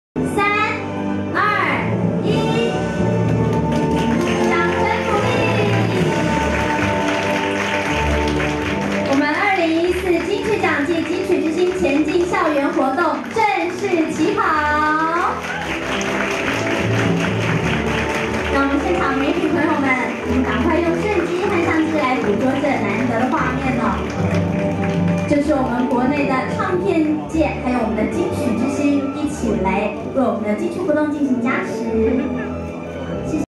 韋禮安 林凡 品冠 郭子 lollipop f 管罄 葉柄還 啟動儀式《第25屆金曲獎活動起跑暨金曲之星前進校園》記者會
第25屆金曲獎活動起跑暨金曲之星前進校園記者會，韋禮安、品冠、郭子、林凡、Lollipop@F等人。